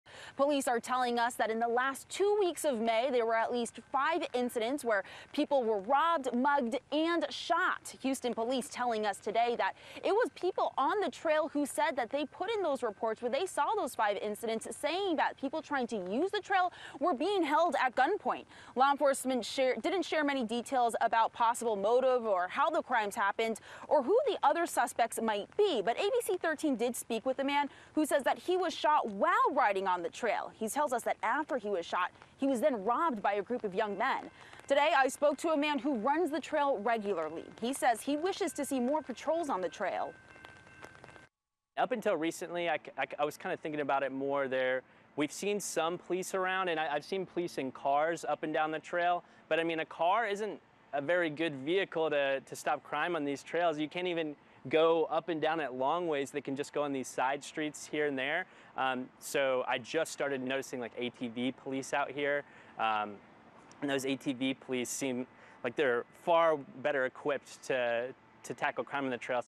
3rd Ward bike trail warning. Police arrest two teens accused of robbing bikers.
3rd Ward bike trail warning. Police arrest two teens accused of robbing bikers.